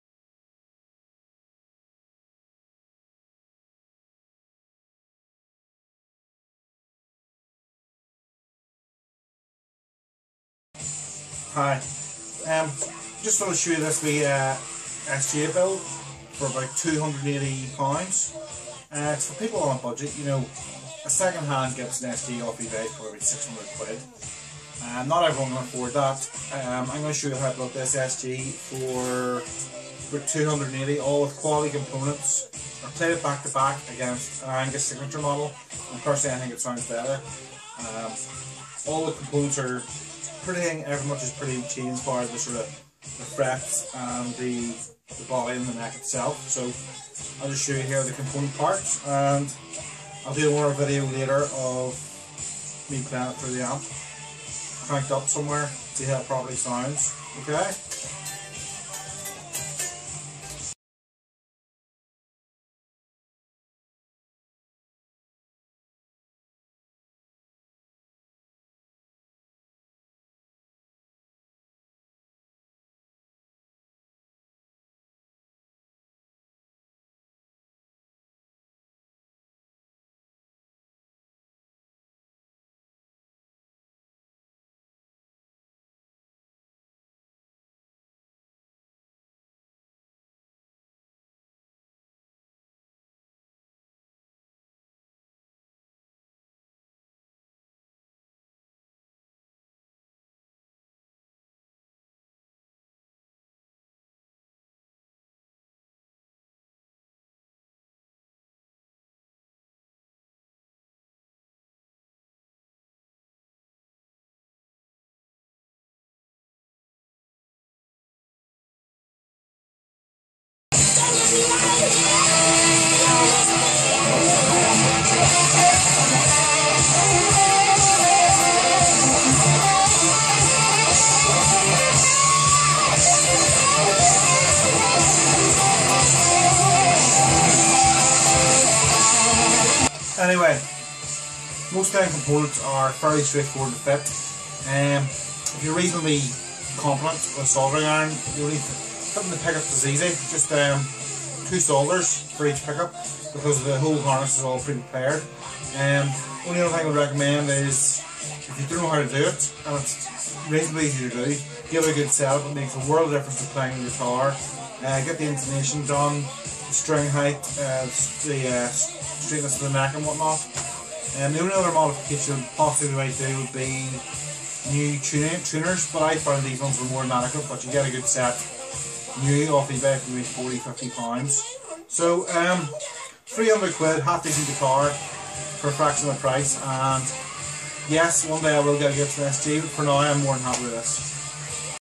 Build a quality SG Guitar for under £300!!!
I built this guitar from a base Westfield E2000 which I believe is made of Mahogany wood. I fitted quality components to create a value for money quality guitar.
I DO NOT advocate the practice of people trying to sell these off as genuine SG'S! This is my main guitar and I plan to keep it.